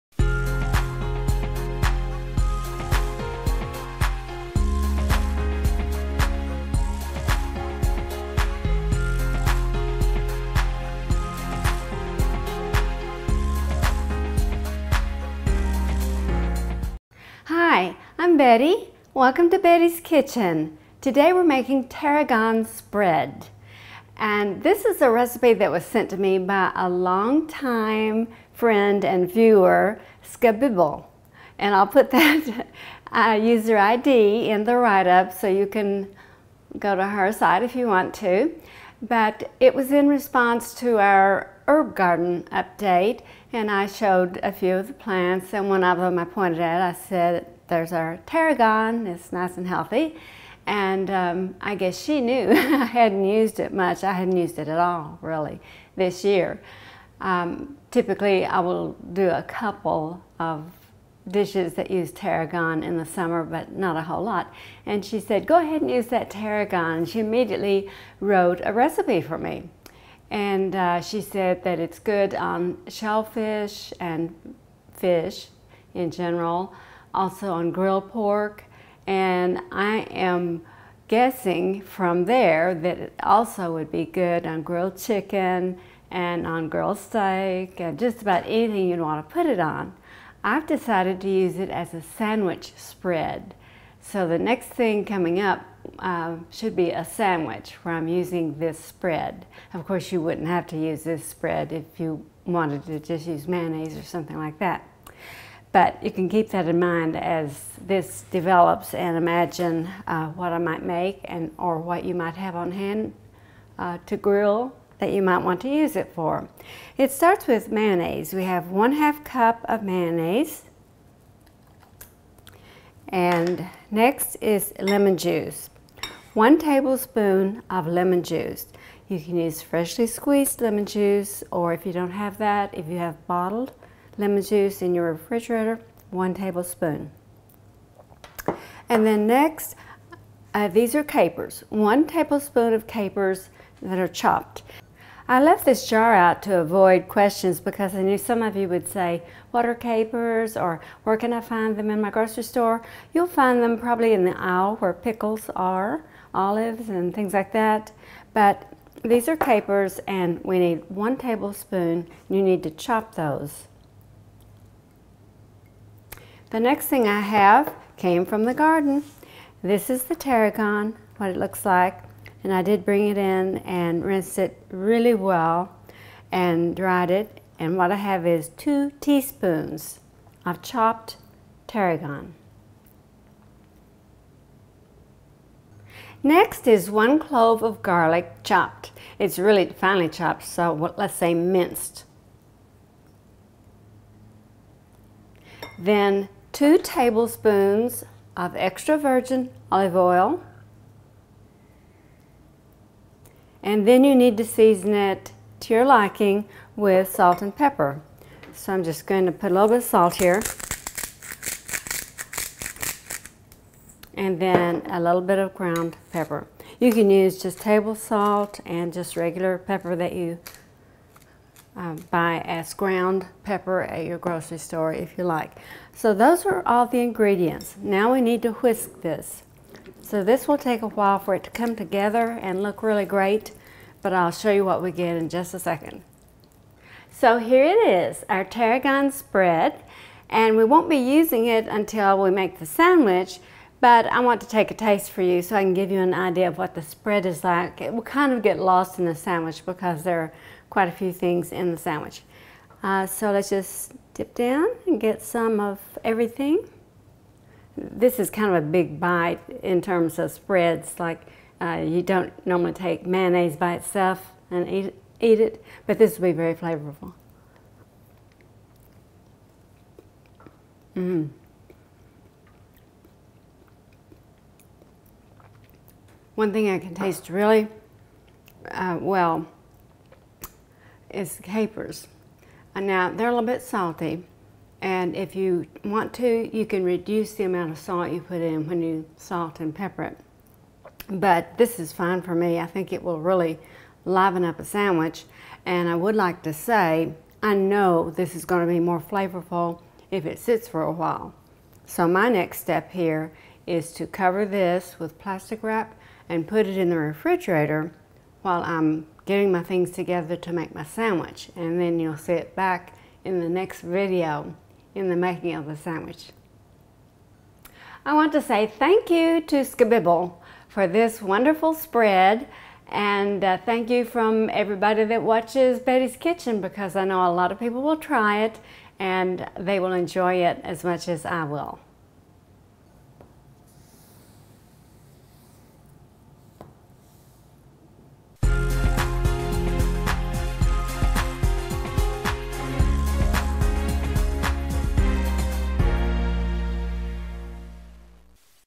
Betty's Tarragon Spread--Recipe by skabibble on YouTube
Betty demonstrates how to make Tarragon Spread.  This recipe was sent to me by a YouTube friend, skabibble.  The spread may be used on fish, shellfish, grilled pork, and other seafood, meats, and poultry.

Tarragon Spread

½ cup mayonnaise
1 tablespoon lemon juice
1 tablespoon chopped capers
2 teaspoons chopped tarragon
1 minced clove garlic
2 tablespoons extra-virgin olive oil

In a small bowl, place ½ cup mayonnaise, 1 tablespoon lemon juice, 1 tablespoon chopped capers, 2 teaspoons chopped tarragon, 1 minced clove garlic, 2 tablespoons extra virgin olive oil, and salt and pepper, to taste.  Whisk until well combined.  Cover with plastic wrap until ready to use.  Use on your favorite seafood, meat, or poultry.  I will be using this as a sandwich spread in the next video—coming up in a couple of days.  Enjoy!  Thanks to skabibble on YouTube for supplying this great flavor combination!  --Betty  :)